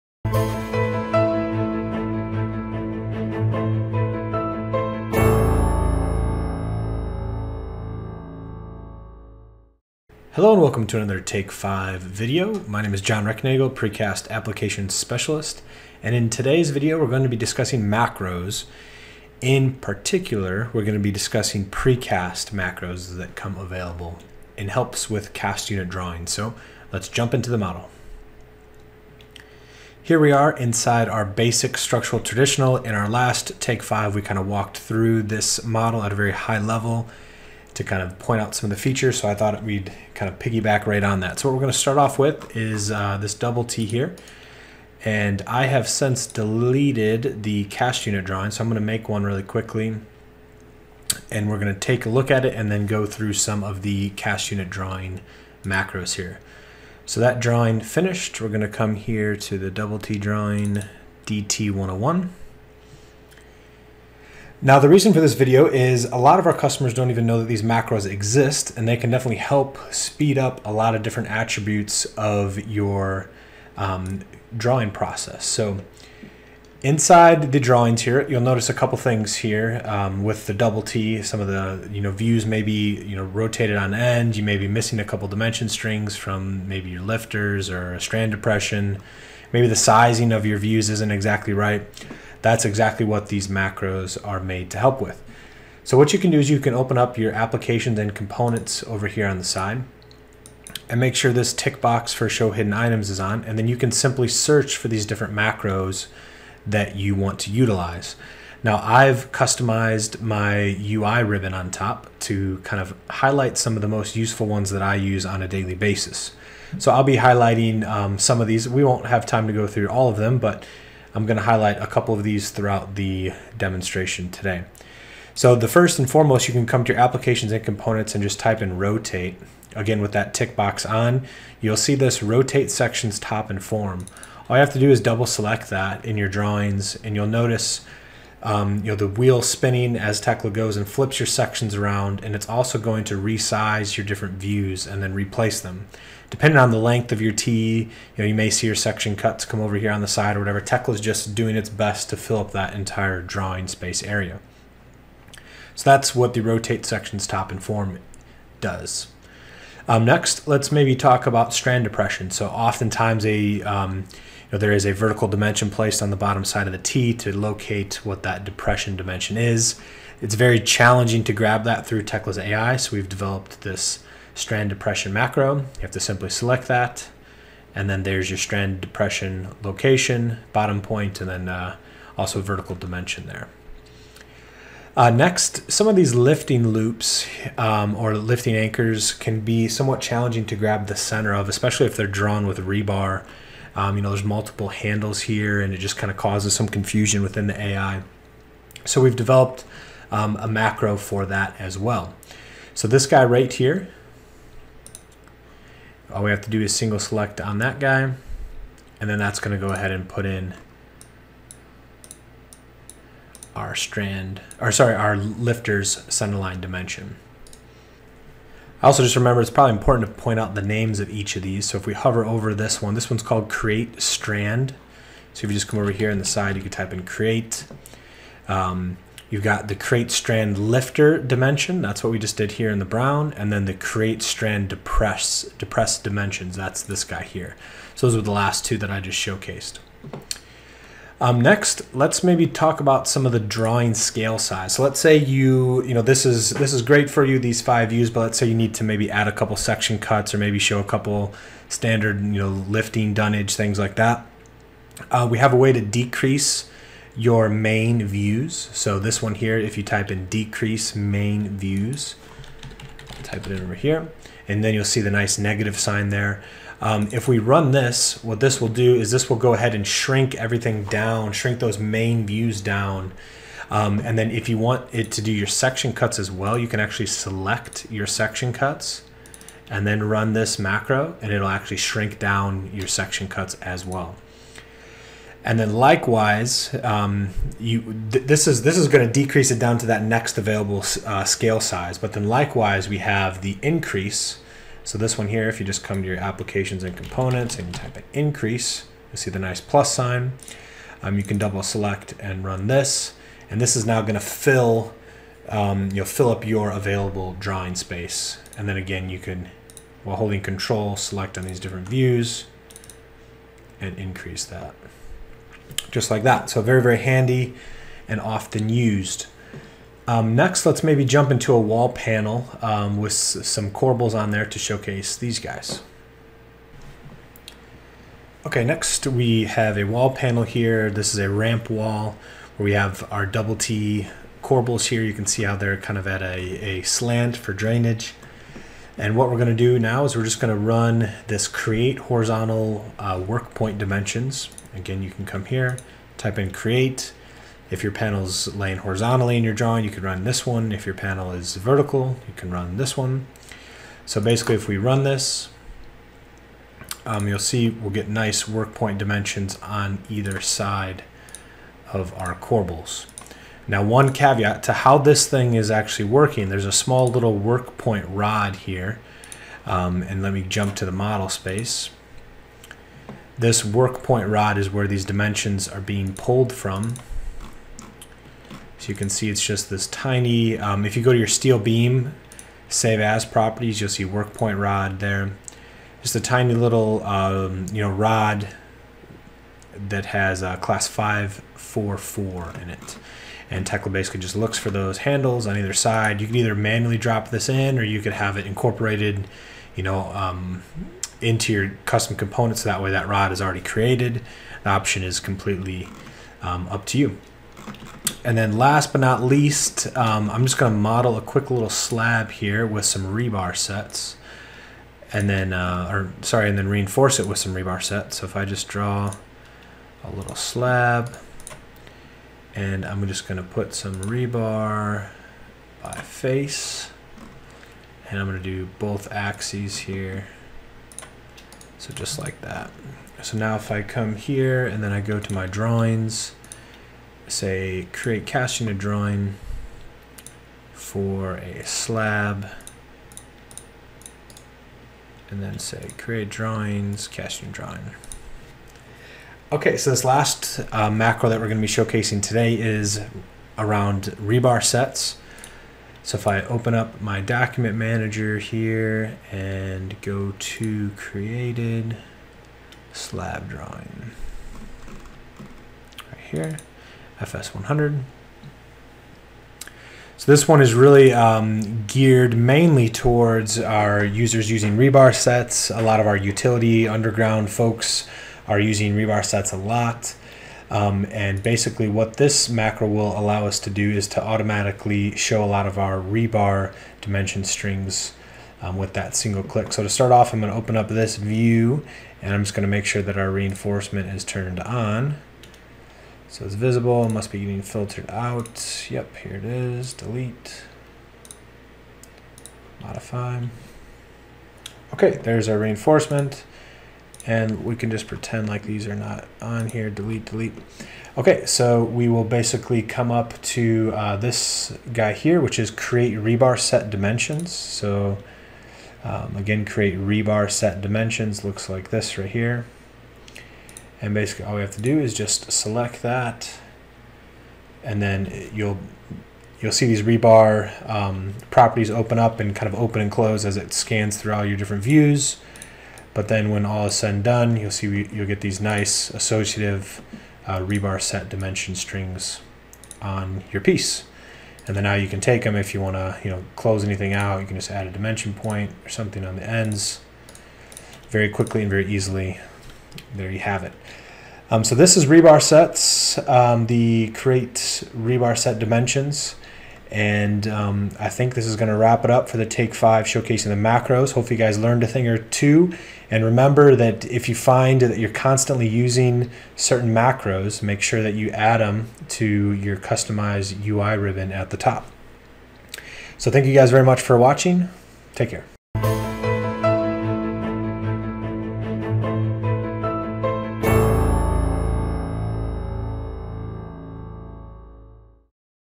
Take Structures Precast CU Drawing Macros
In this video, we take a look at some of the cast unit drawing macros available for the precast role.  Watch this video to see how macros can help improve your productivity and workflow processes.